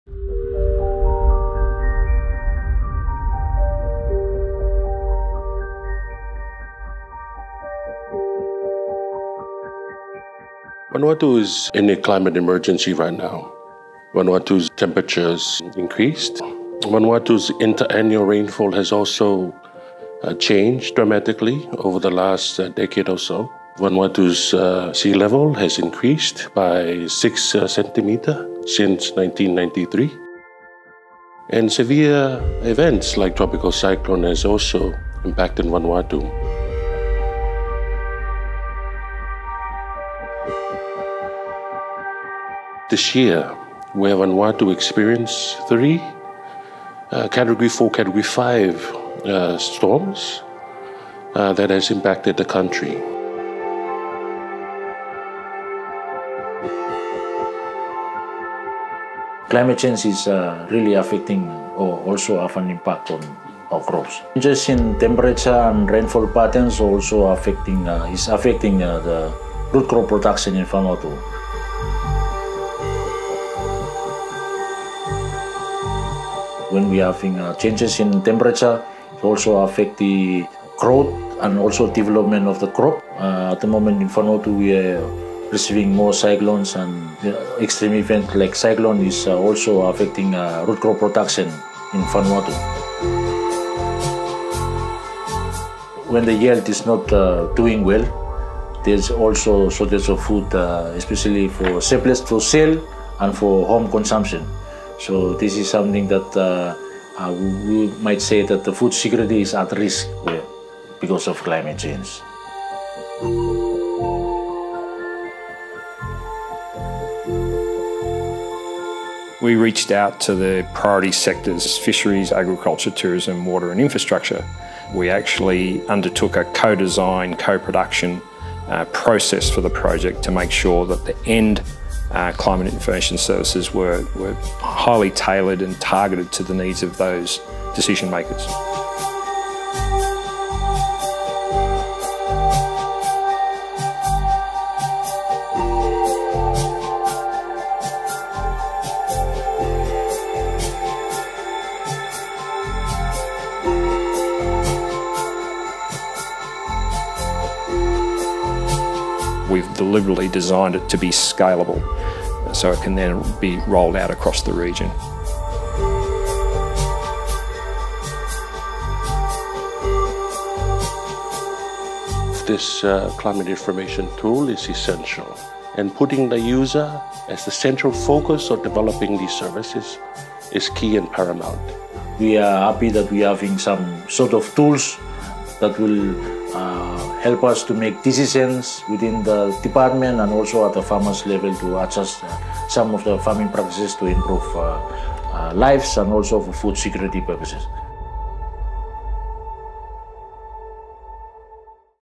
New game-changer climate information tool for Vanuatu
The new Climate Futures Portal, delivered as part of the Climate Information Services For Resilient Development In Vanuatu (Van-KIRAP) project, in collaboration with the Vanuatu Meteorology and Geo-hazards Department (VMGD), and the Secretariat of the Pacific Regional Environment Programme (SPREP), is now available.

The Vanuatu Climate Futures Portal includes global and regional climate projections (at varying spatial scales) for multiple climate hazards including rainfall, temperature, sea level rise, tropical cyclones (including extreme winds), marine heatwaves and ocean acidification. The information is helping inform adaptation, planning and support decision making for agricultural, infrastructure, fisheries, tourism and water industries.

We learnt a lot from this unique collaboration around best to develop, apply, and deliver tailored and targeted climate information.